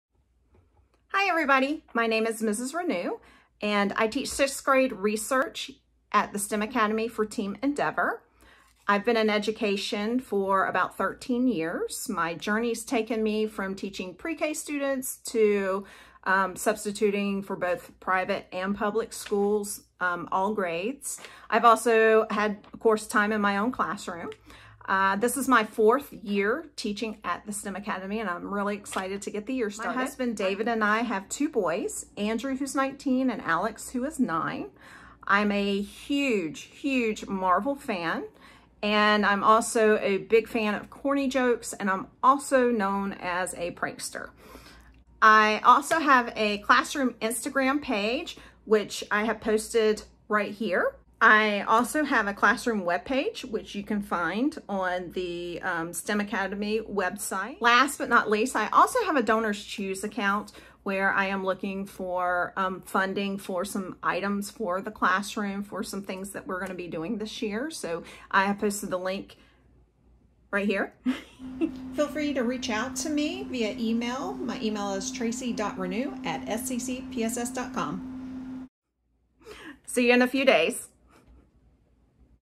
WelcometoSTEMSAV - Meet Ms. Ranew
Welcome to The STEM Academy! Meet Ms. Ranew, part of the Sixth Grade team.